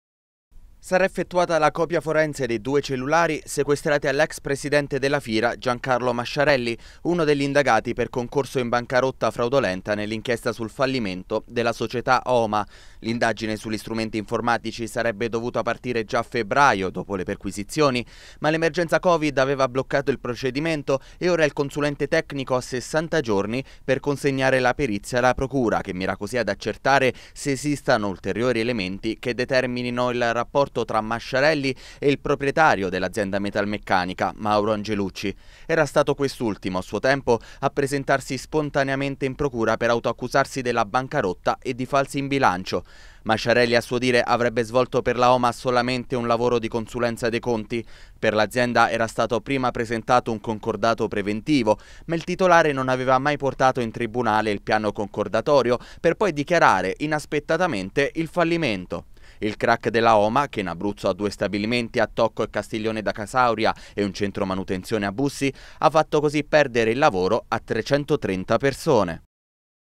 Crack Oma. Attesa perizia sui cellulari di Masciarelli
La procura mira ad accertare il rapporto tra Giancarlo Masciarelli e Mauro Angelucci nell'inchiesta sulla bancarotta della Oma. Entro 60 giorni è attesa la perizia sui due telefoni cellulare dell'ex presidente della Fira.